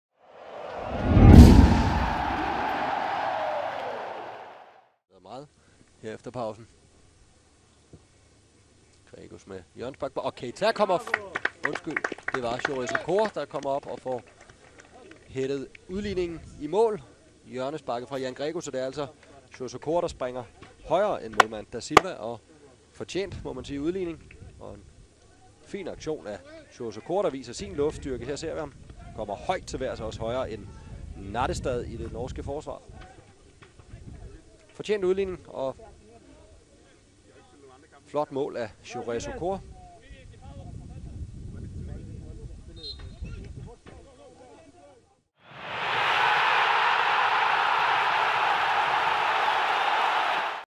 Se Okores første FCK-mål
Jores Okore scorede i går sit første mål for løverne siden skiftet fra Aston Villa i træningskampen mod Molde. Se scoringen her - ses vi mod Brøndby næste søndag i Telia Parken?

profilnavn: fc_kobenhavn